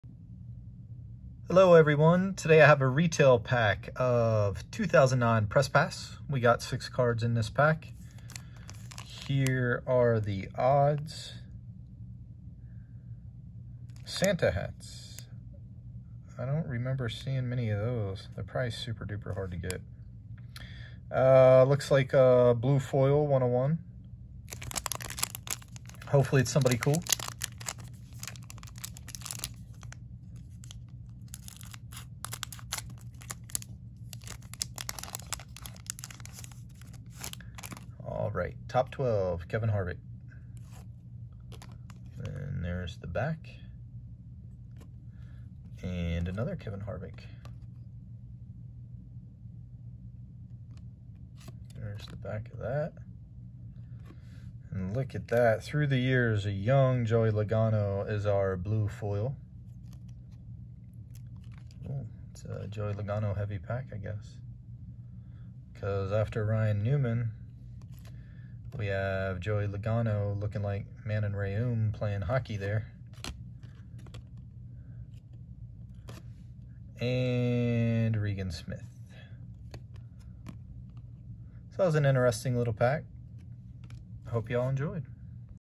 Press Pass 2009 NASCAR pack rip
I open 1 retail pack of #2009 Press Pass nascar racingcards hope you enjoy!